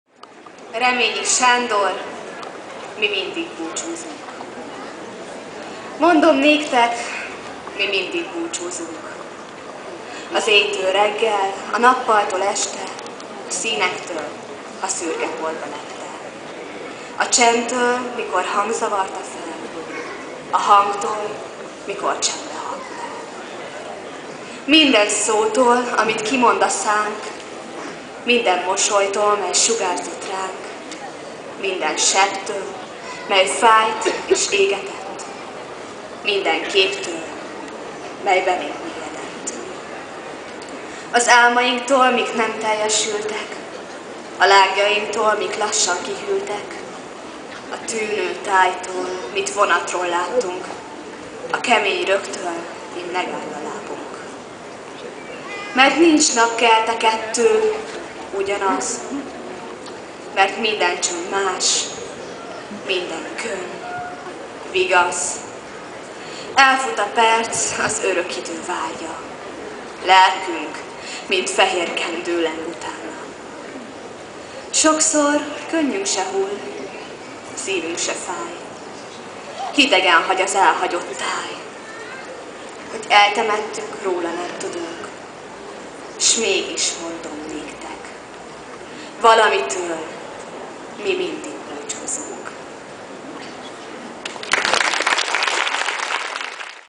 Ballagás 2011 - 07 - Reményik Sándor: Mi mindig búcsúzunk (Sponga Beáta)
A Semmelweis Ignác Humán Szakképző Iskola és Gimnázium 2011-es ballagási ünnepségén készült videófelvétel